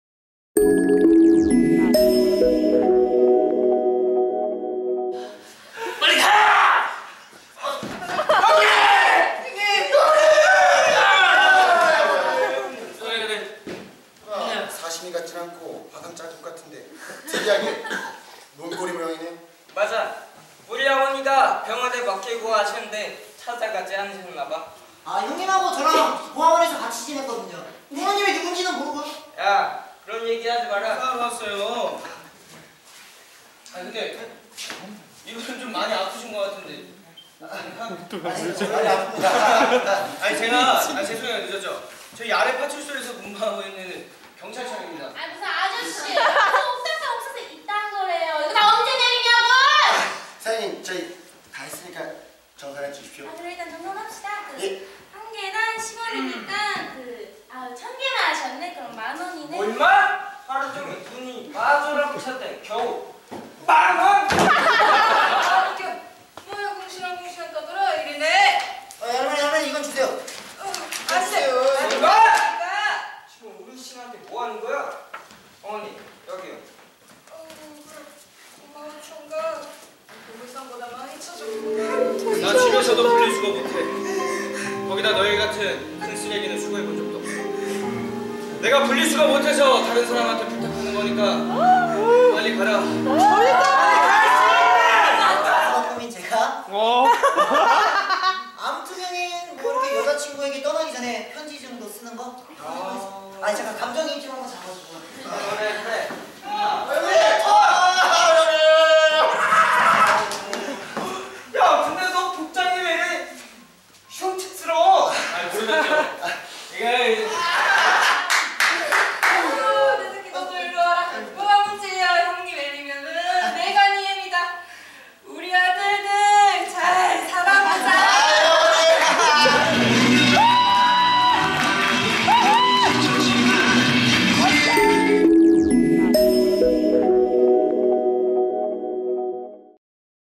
[싹튜브] 30th SAC아트페스티벌 개그시트콤전공 옥탑방 천사들 지도 표인봉,유기정,신대호 교수, 연출 정요셉 서울종합예술실용학교
30th SAC아트페스티벌 개그시트콤전공 옥탑방 천사들 지도 표인봉,유기정,신대호 교수, 연출 정요셉